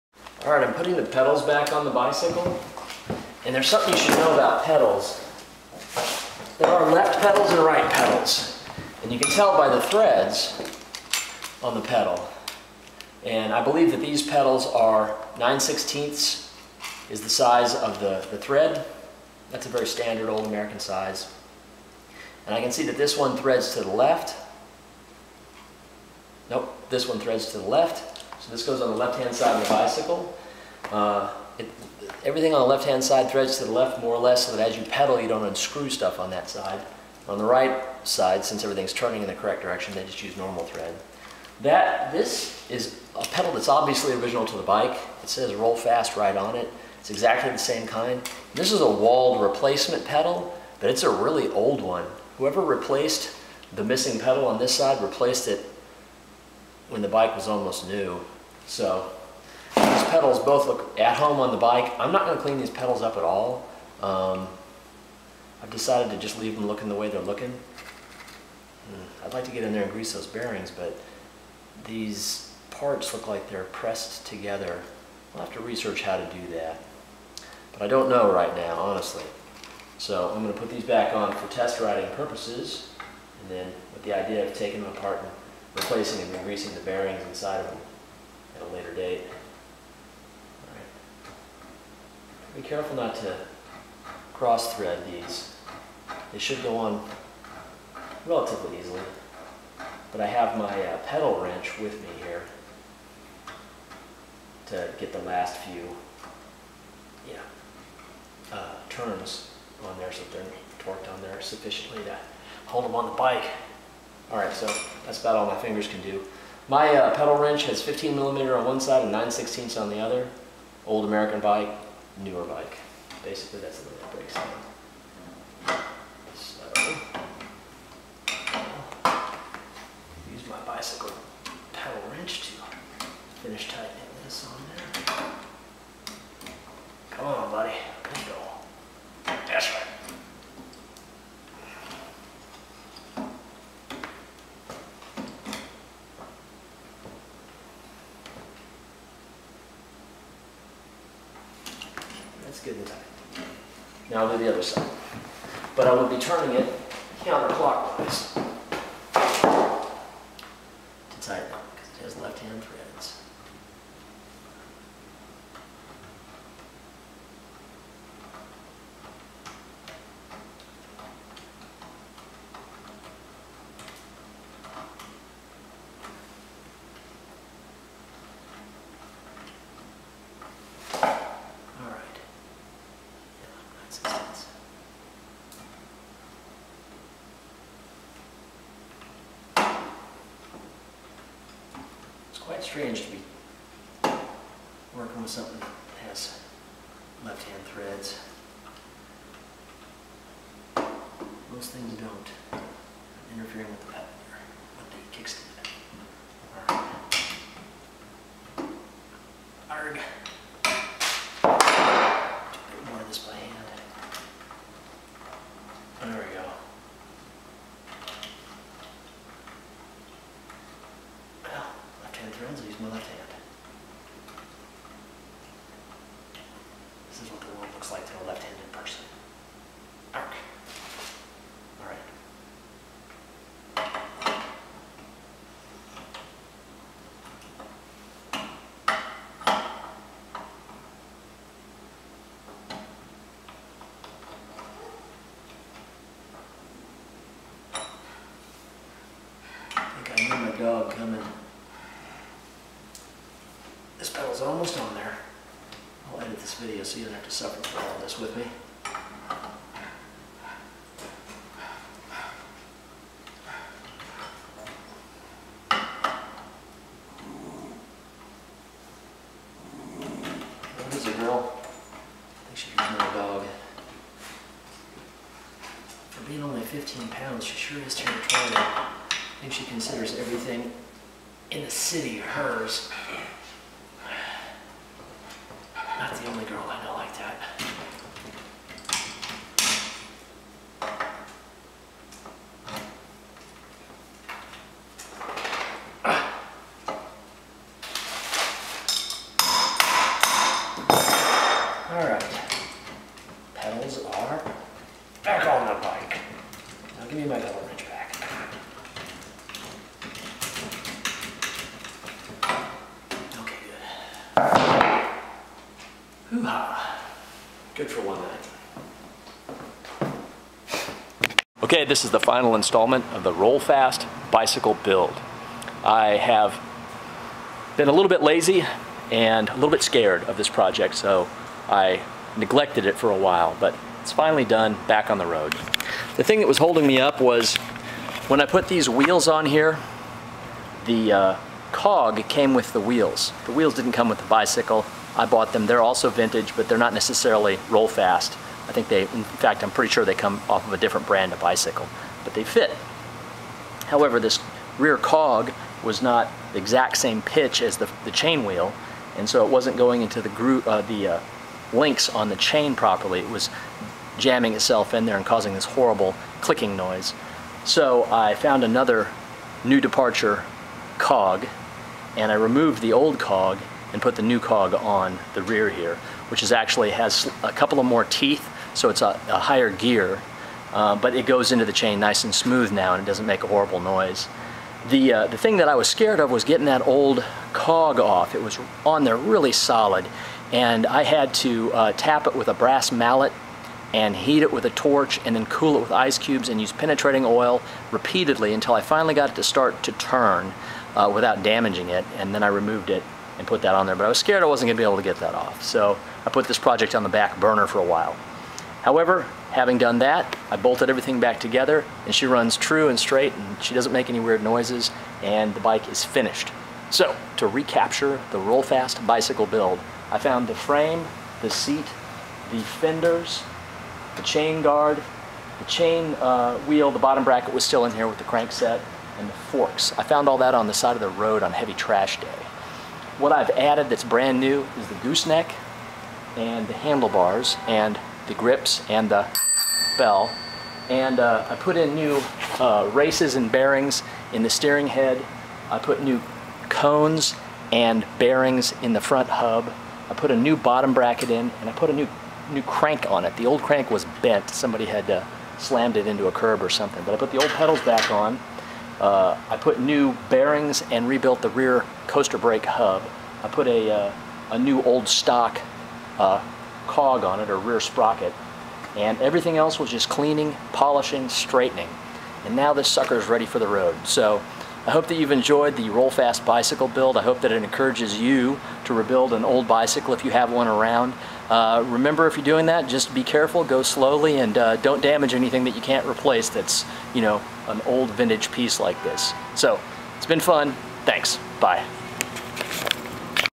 Rollfast Part 11
Part eleven, the final installment, of my rebuild of a 1948 Rollfast vintage American cruiser bicycle.